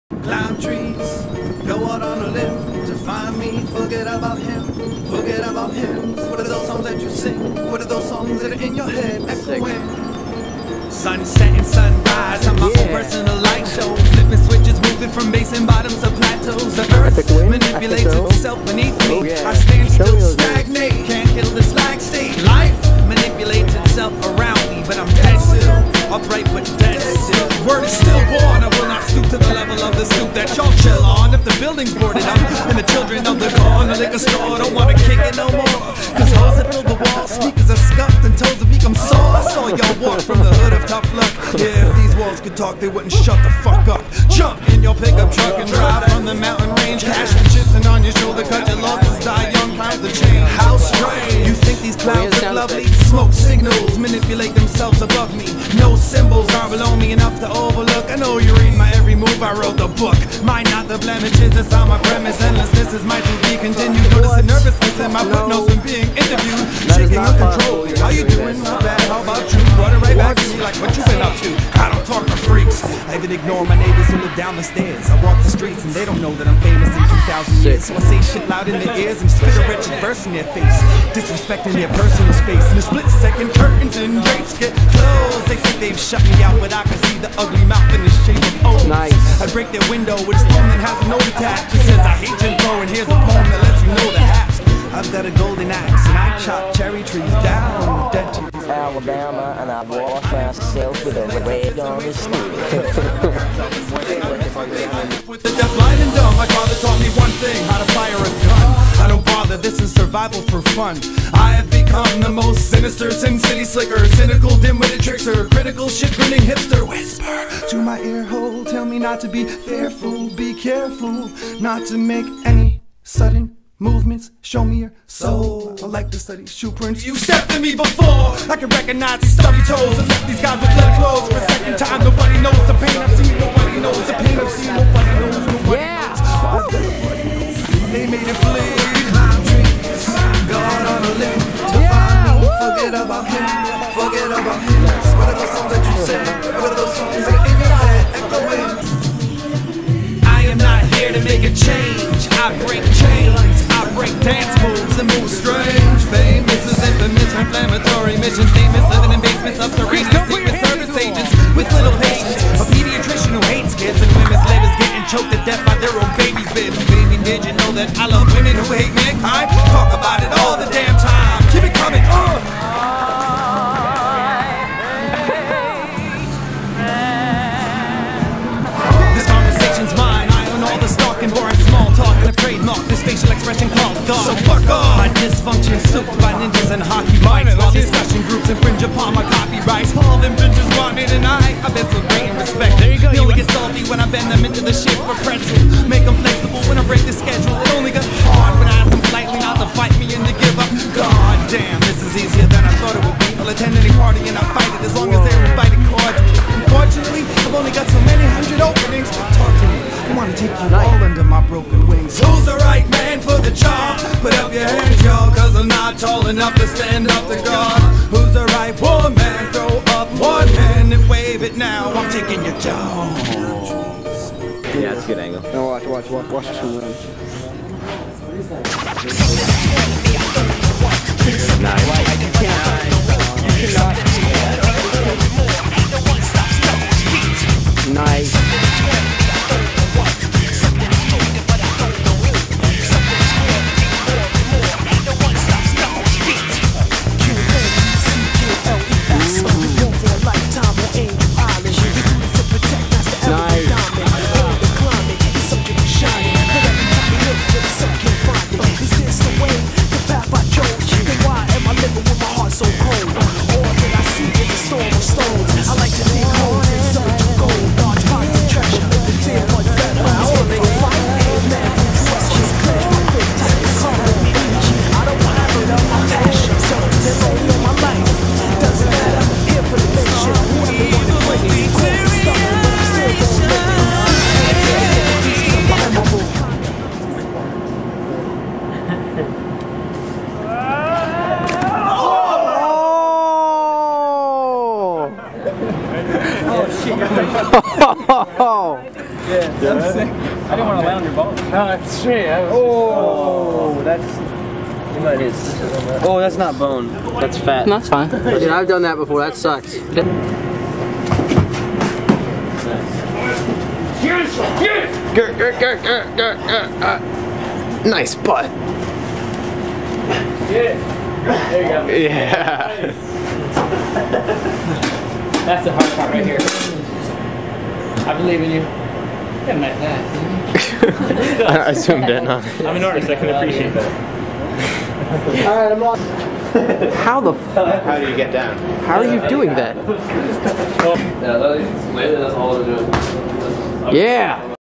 Parkour Shenanigans! W/ Ryan Doyle, Danny Arroyo, Tom Coppola, Brian Prince, Marvin Ross +
I met some pretty cool people on my parkour journey